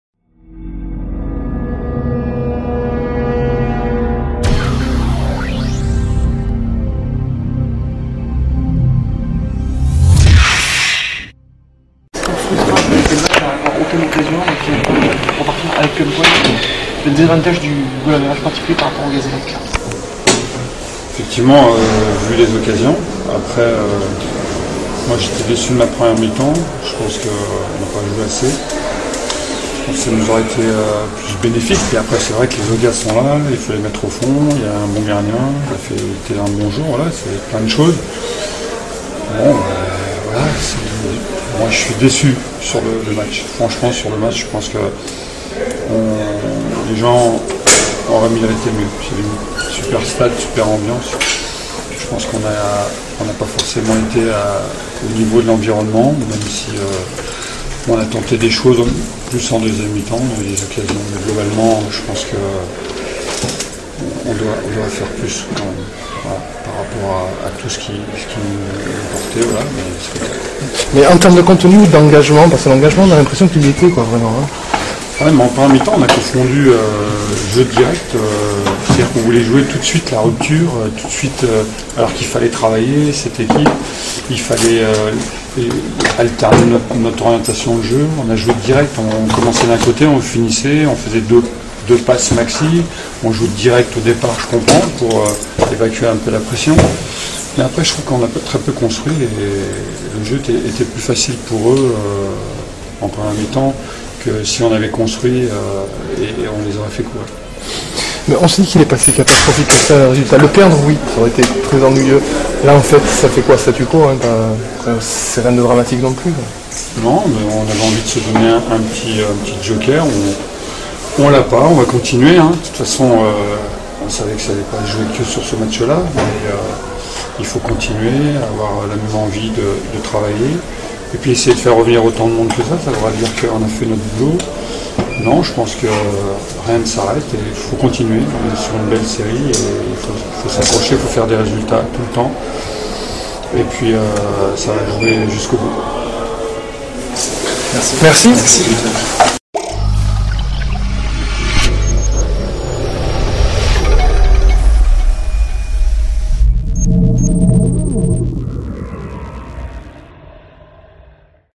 Réactions de Thierry FROGER (Nîmes-Ajaccio)
Réactions du coach Thierry FROGER après la rencontre Nîmes / Ajaccio (0-0)